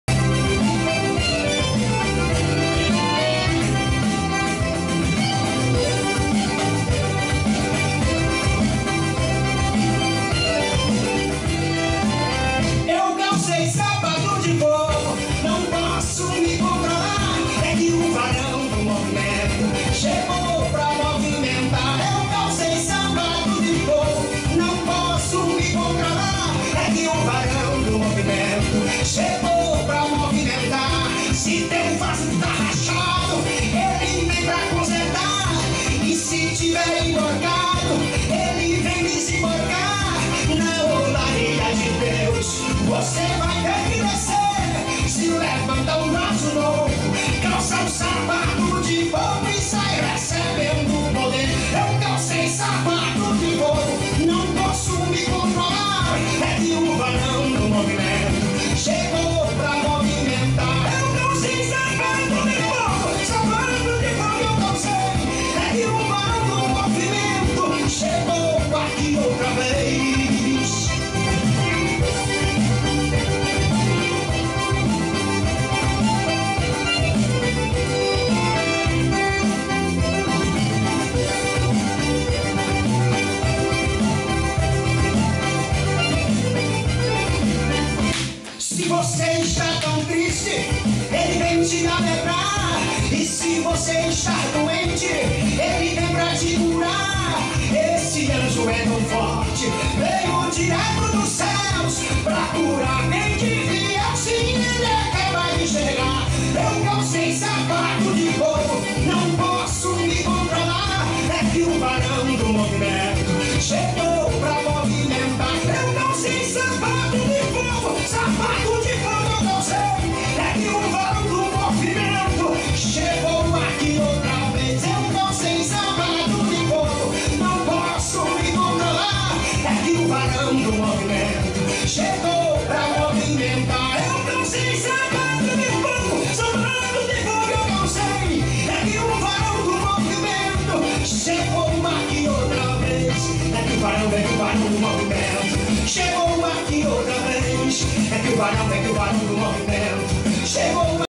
ANA MARIA DANÇANDO SAPATO DE FOGO
ESTE VÍDEO MOSTRA A DANÇA DO SAPATO DE FOGO E OLHA QUE A DANÇARINA VAI COMPLETAR 70 ANOS